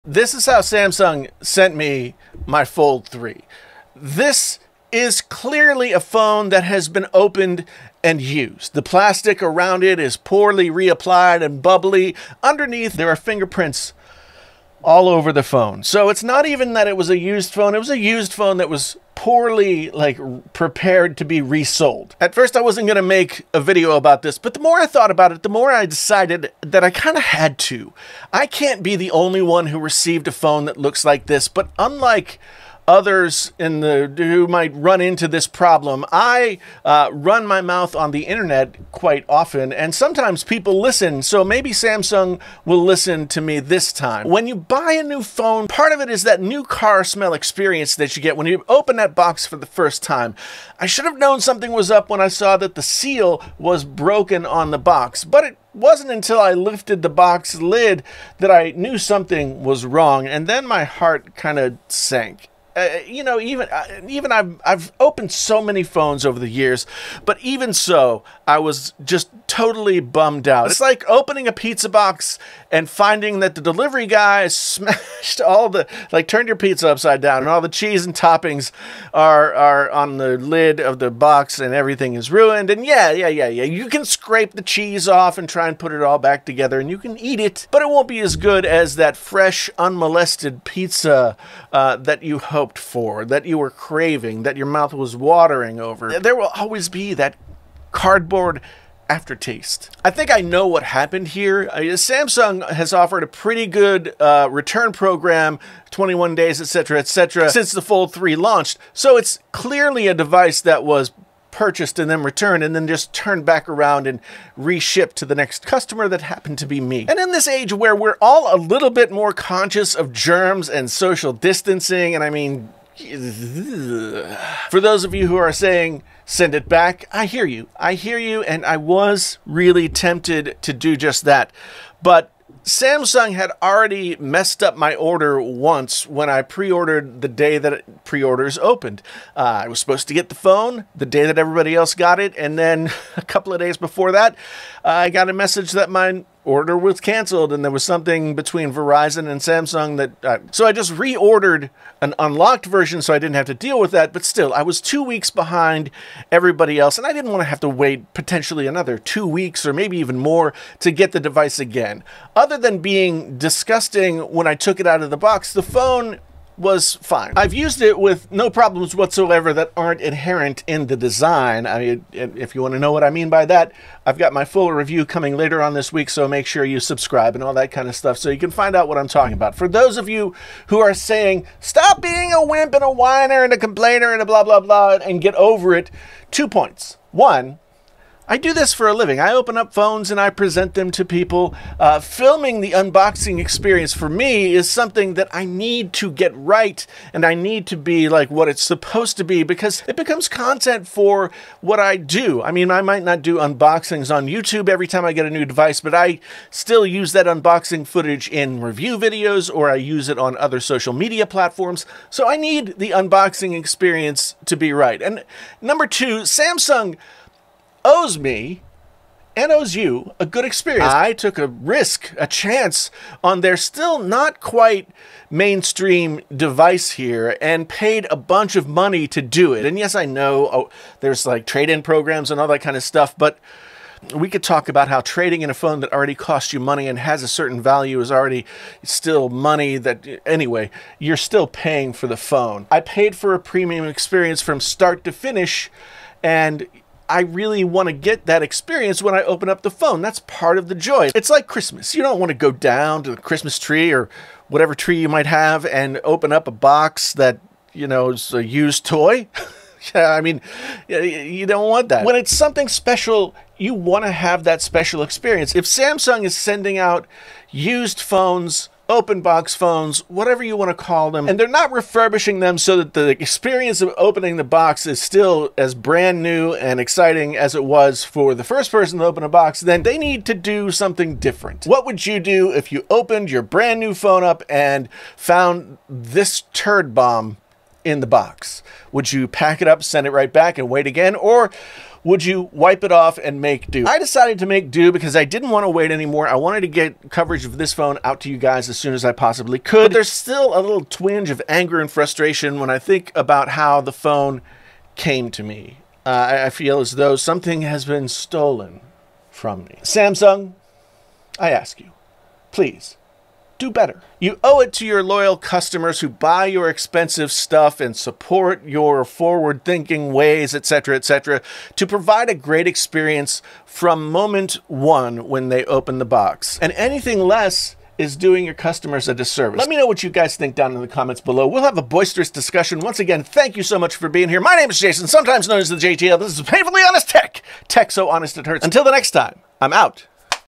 Samsung Sent Me a USED Z FOLD 3!?!?! | Painfully Honest Tech
Samsung sent me a used Zfold 3. I didn't order a used Z Fold 3. I ordered a new Z Fold 3. What the heck, Samsung?